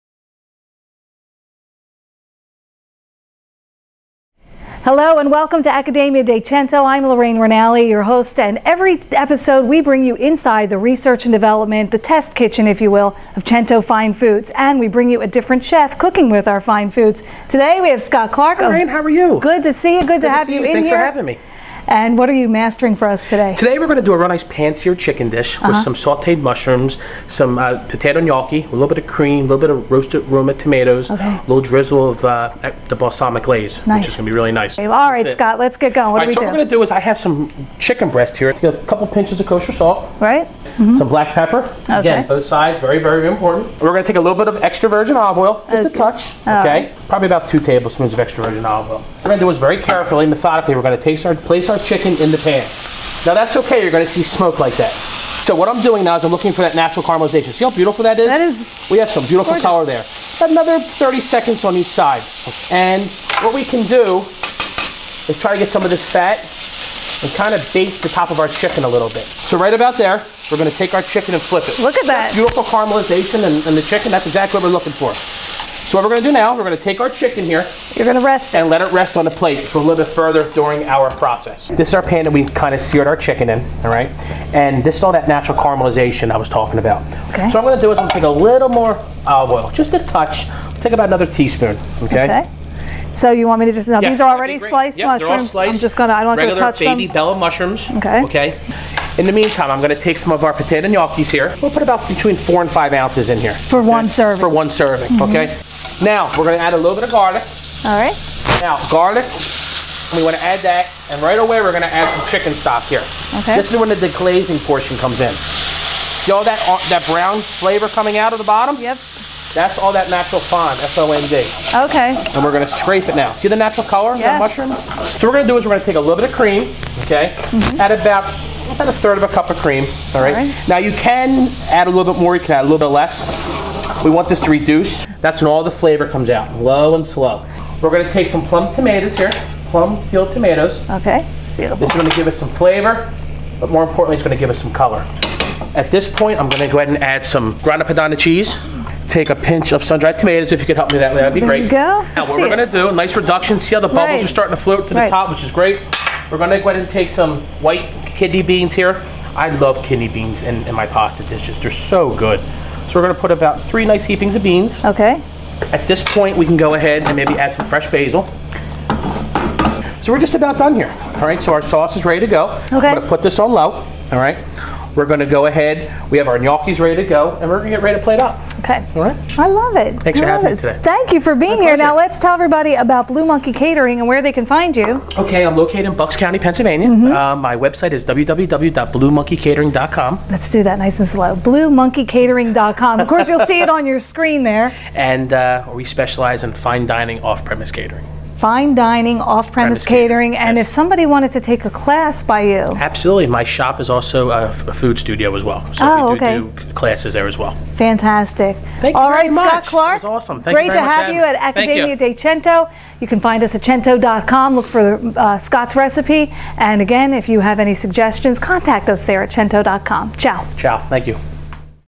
Smoked Chicken & Wild Mushroom Gnocchi by Chef Scott Clarke
Chef Scott Clarke stops by the ADC kitchen to demonstrate how to prepare Smoked Chicken & Wild Mushroom Gnocchi